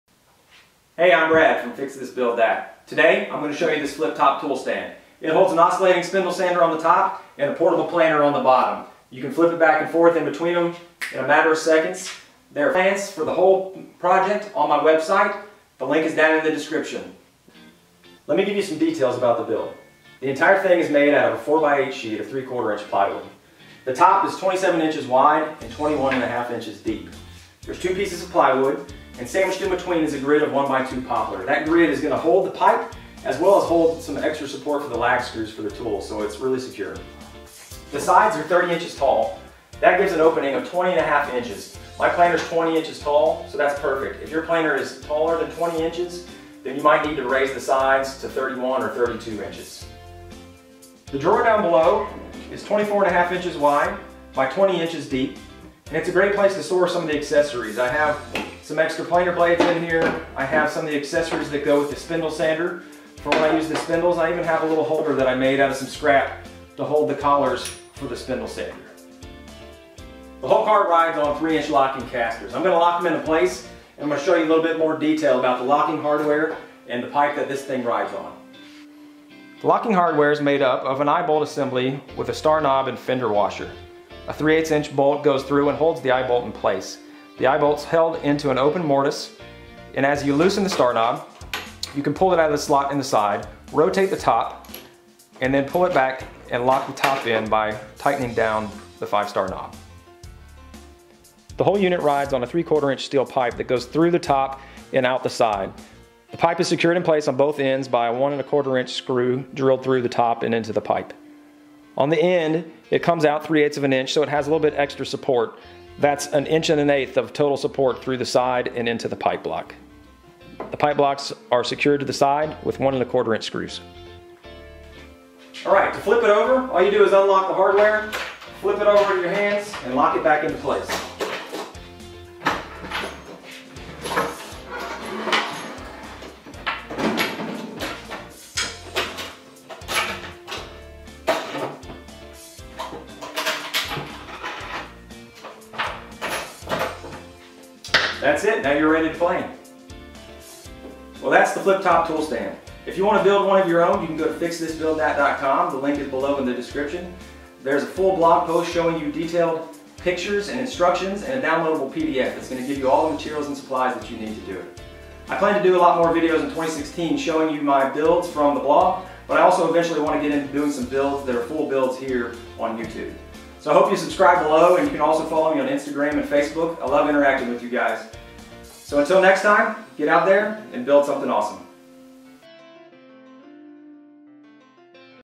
Flip Top Tool Stand for Planer & Spindle Sander | DIY Woodworking Storage
This Flip Top Tool Stand is the perfect station for your planer, sander, miter saw, or any other benchtop tool.  Shop workstations should serve double duty and this one does!